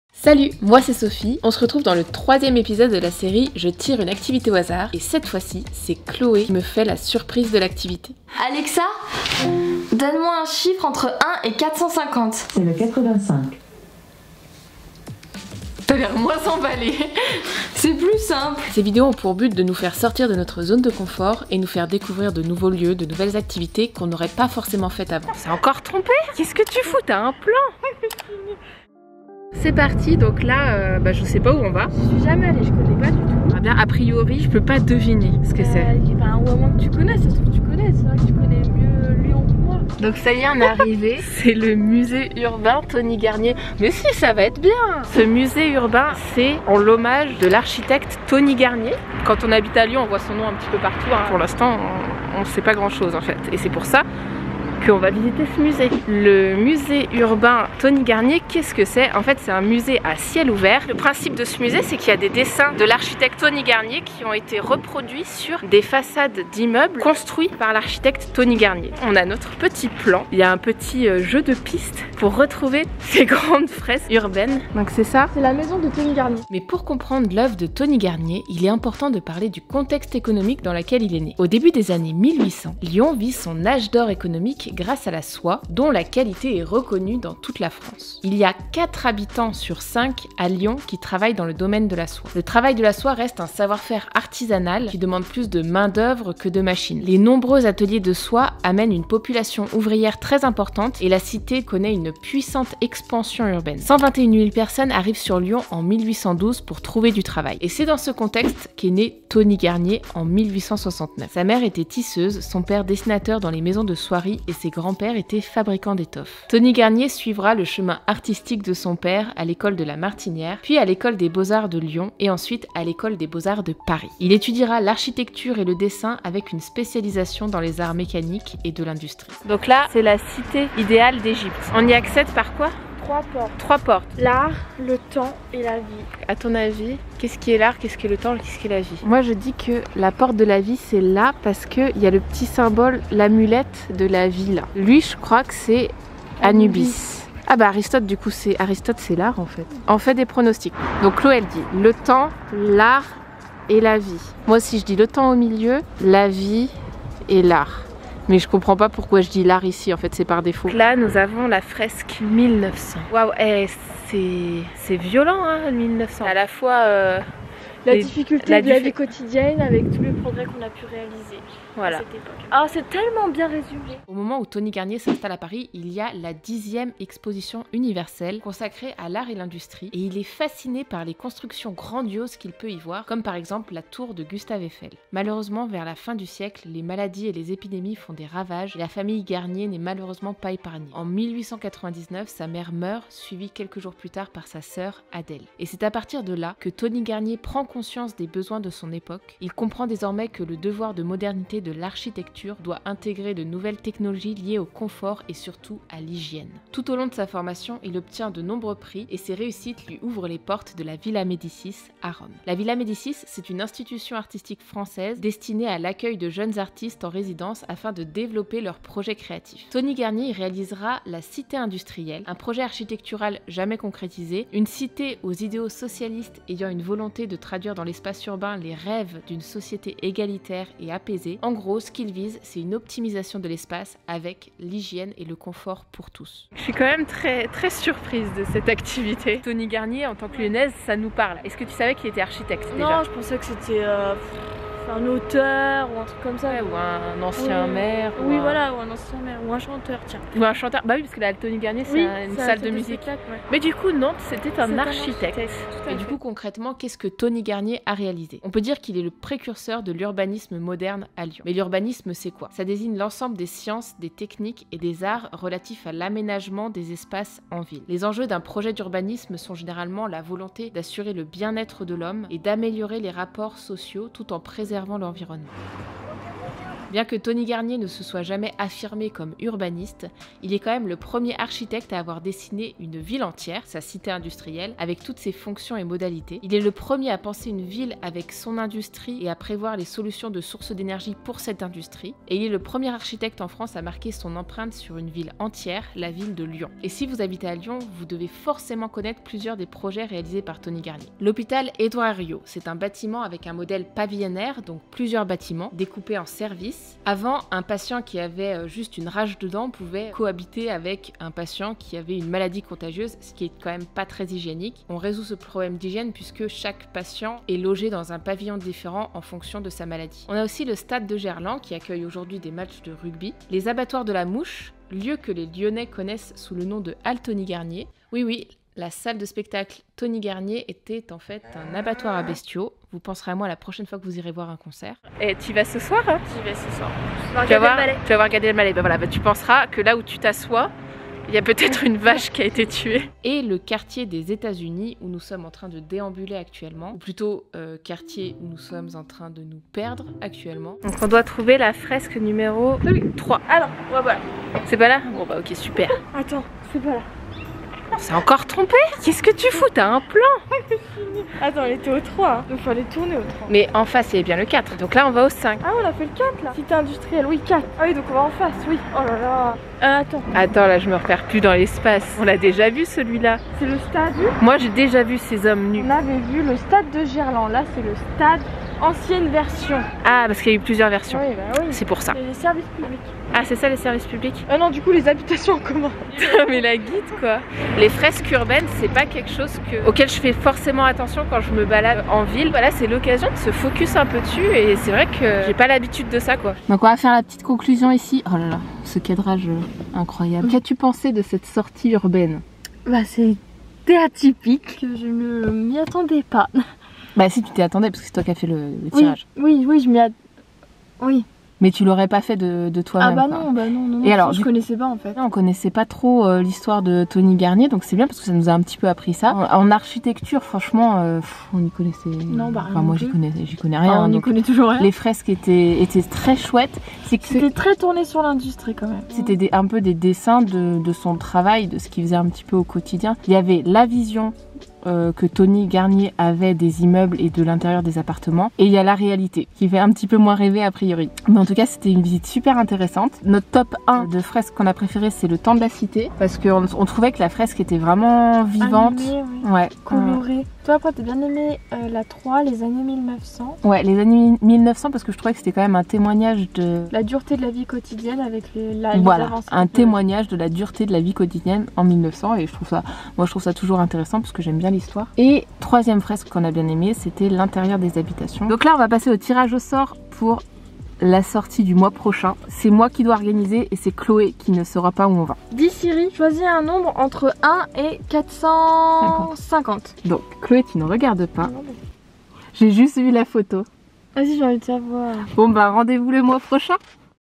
Vlog au musée urbain TONY GARNIER (activité au hasard #3)
Tony Garnier, en tant que lyonnaises, on connait de nom. Mais on ne savait pas qu'il était architecte et à l'origine de nombreux bâtiments à Lyon.

Et si vous voulez trouver d'autres activités trop cool à faire